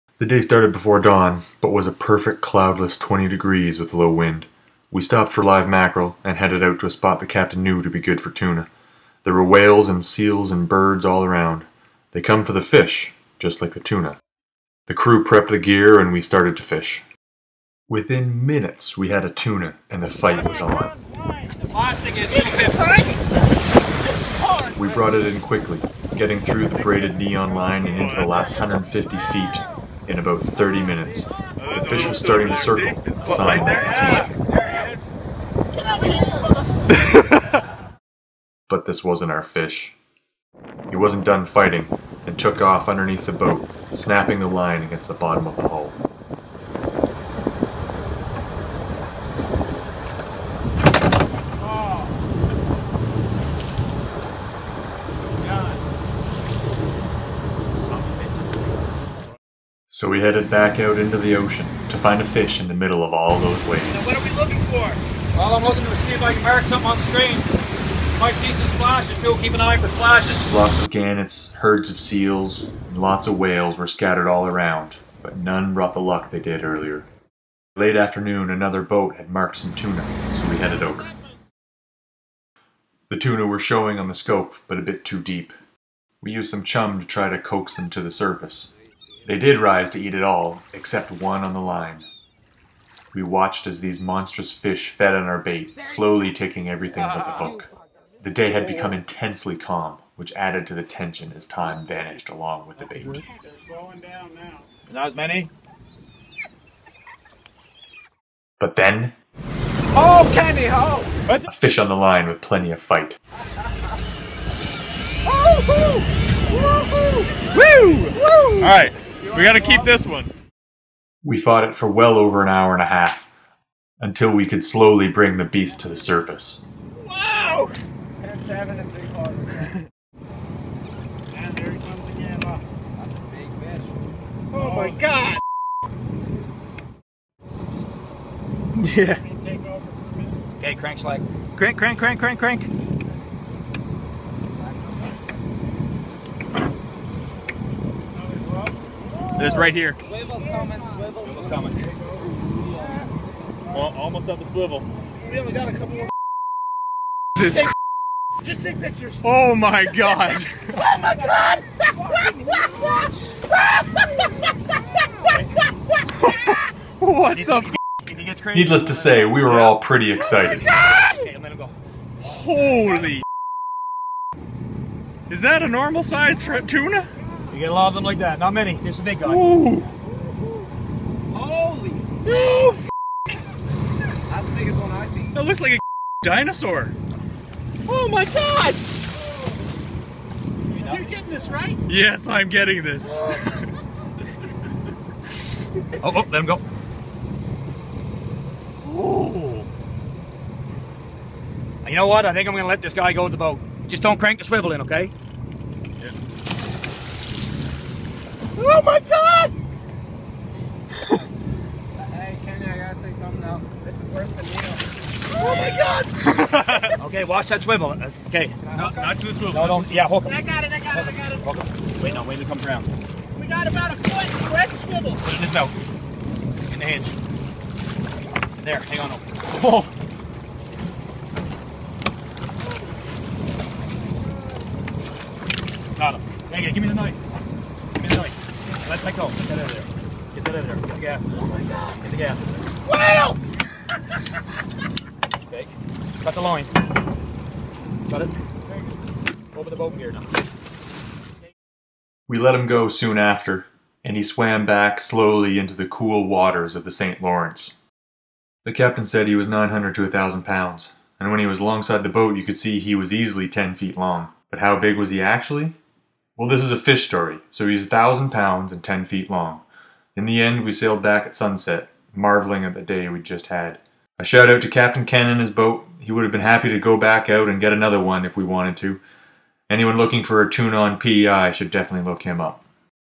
PEI Tuna fishing adventure
A summary video of our epic tuna charter in PEI.  We had a perfect day, and lots of excitement.  If you're not interested in the full story, jump to the 2:40 mark to see how it ended.

Big thanks to Kenny McRae and his crew for taking us out. to get into it yourself.